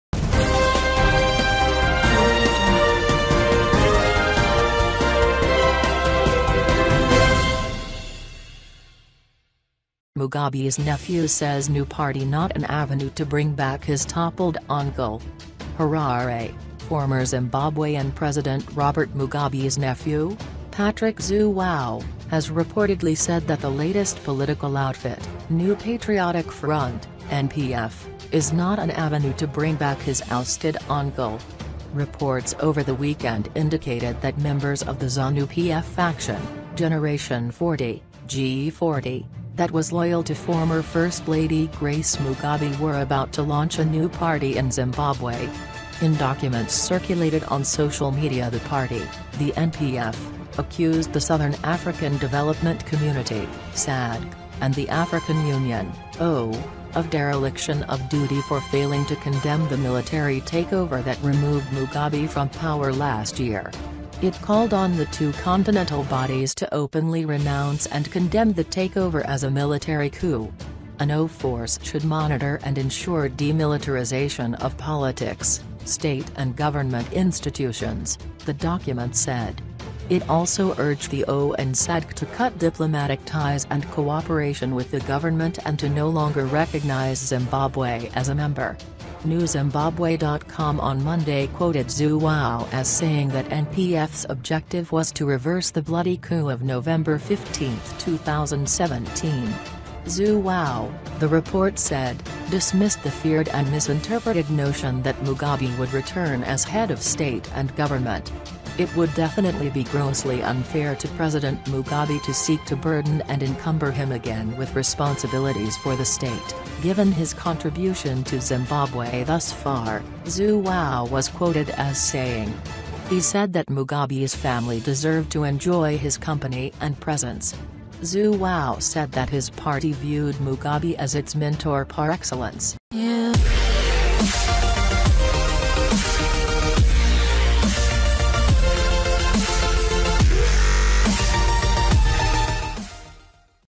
Patrick Zhuwao says new party not an avenue to bring back his toppled uncle
Harare – Former Zimbabwean president Robert Mugabe's nephew, Patrick Zhuwao, has reportedly said that the latest political outfit, New Patriotic Front (NPF), "is not an avenue to bring back" his ousted uncle.
Reports over the weekend indicated that members of the Zanu-PF faction, Generation 40 (G40) that was loyal to former first lady Grace Mugabe were about to launch a new party in Zimbabwe.
In documents circulated on social media the party, the NPF, accused the Southern African Development Community (SADC) and the African Union (AU) of "dereliction of duty" for failing to condemn the military takeover that removed Mugabe from power last year.